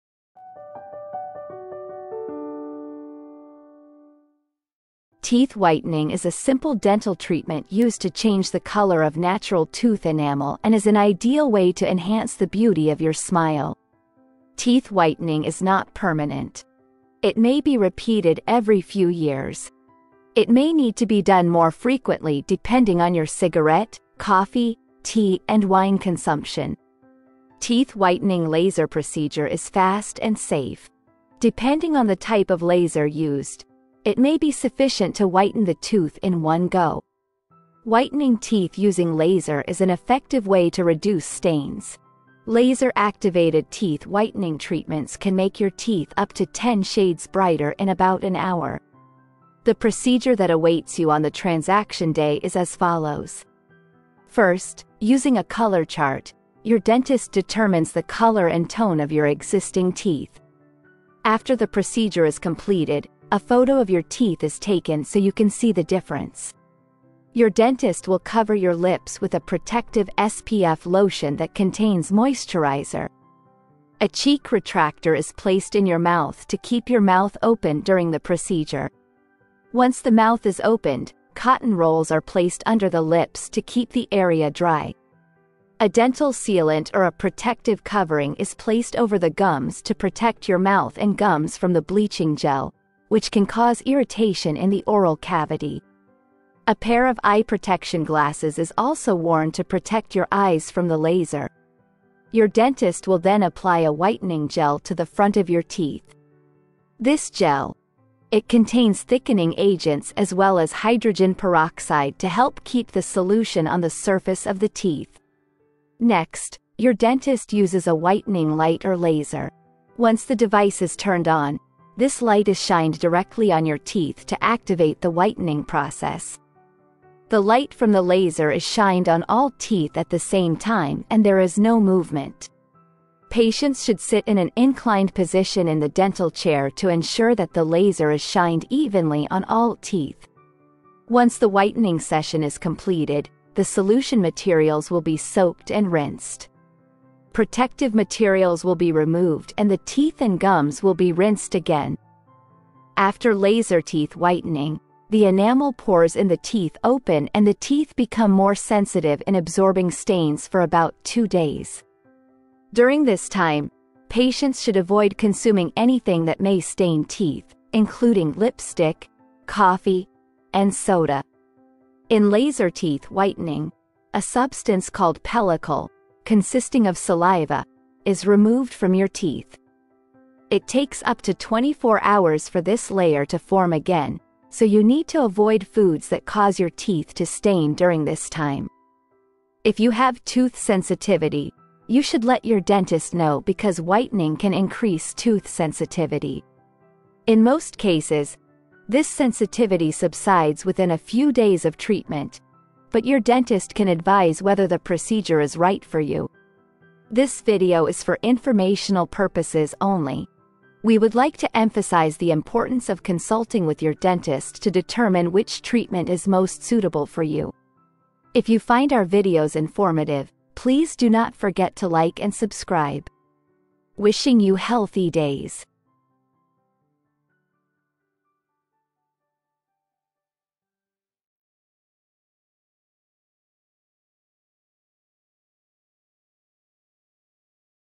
How is Teeth Whitening Laser Procedure Done?
In this video, we provide detailed information about the teeth whitening process. Teeth whitening is an effective method used to change the color of natural tooth enamel and can significantly improve your smile.

In our video, we explain step by step how to whiten teeth. It is important to decide with your dentist which treatment option is best for you, so be sure to consult with a dentist before the procedure.

Teeth whitening may be repeated every few years, depending on the consumption of stain-causing substances such as cigarettes, coffee and tea. Laser teeth whitening is a fast and effective method and can often provide significant results even in a single session.

In our video, we also give detailed information about what you should pay attention to during the procedure and what you should pay attention to after the procedure. Things to consider after teeth whitening can help ensure the long-lasting effect of the procedure.